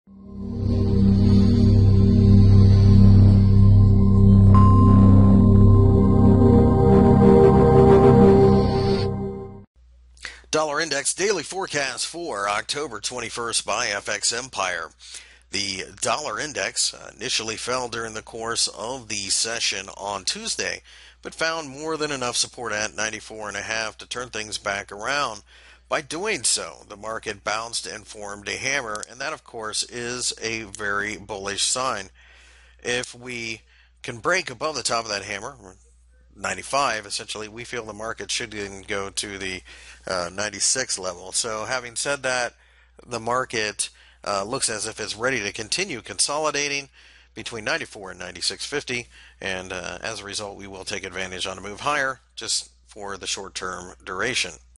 US Dollar Index Technical Analysis for October 21 2015 by FXEmpire.com
- October 21 2015 index daily technical analysis for the US Dollar INDEX. Find more information about Forex and Commodities News Technical Analysis Fundamental Analysis and Brokers on FX Empire